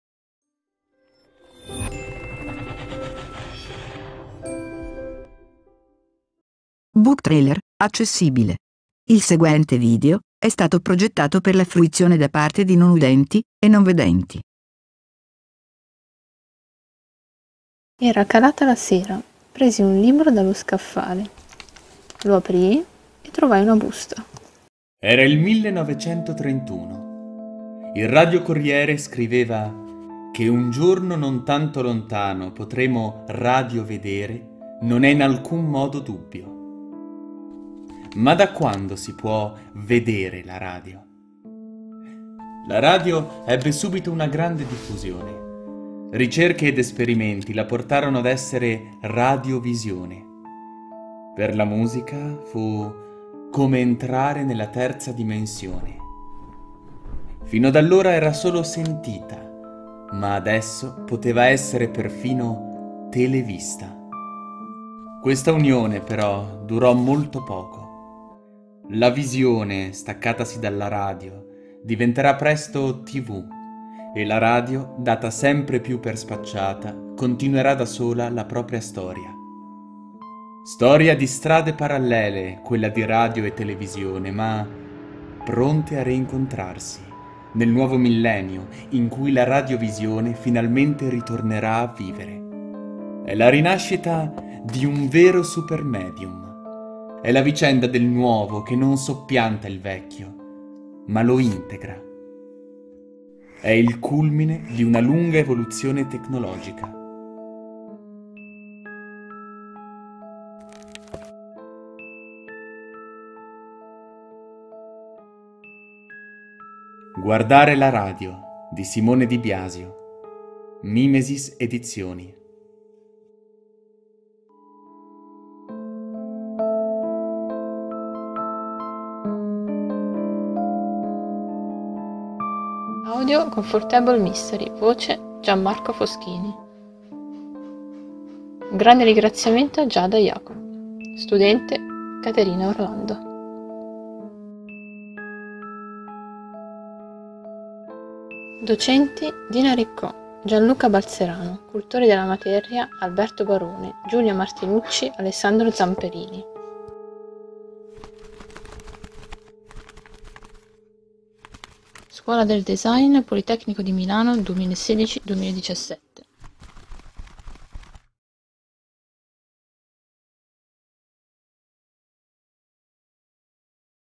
Booktrailer Accessibile - Guardare la radio - Mimesis Edizioni
Booktrailer realizzato per  un  corso presso il Politecnico di Milano.